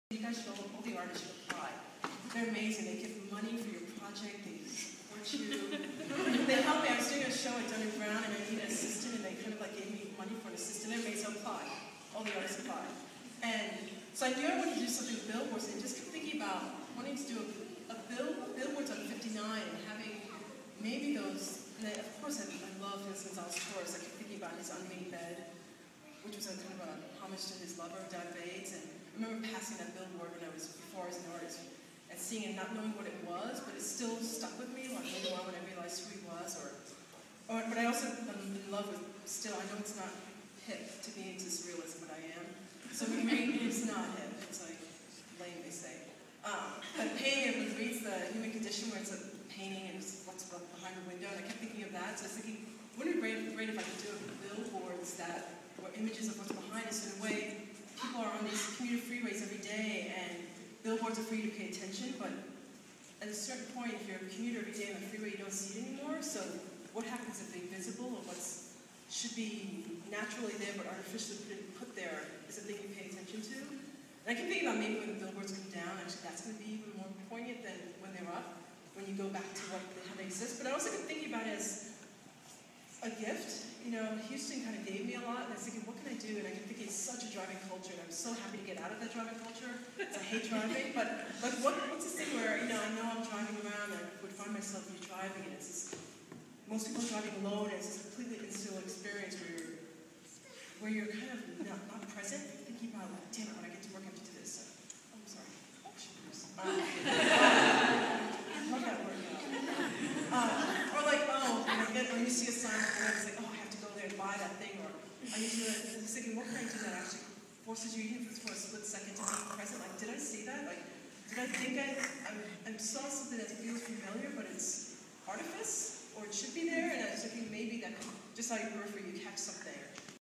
Karen Olivier - Artist Talk for inbound at DiverseWorks pt2
The Mitchell Center is proud to announce a bold new project from artist-in-residence Karyn Olivier.  Using 10-15 billboards along the citys downtown freeways, Olivier will replace the usual advertisements with images of the urban landscape that would be visible if the billboard did not exist - the sky, trees, and buildings obstructed by the ads will now be revealed. Presented in collaboration with the Moores School of Music, DiverseWorks, and CBS Outdoor.

Olivier will also work with UH students enrolled in the Mitchell Centers IART course Mitchell Center Mentorships, taught by artist J. Hill, to produce individual and collaborative projects in the community.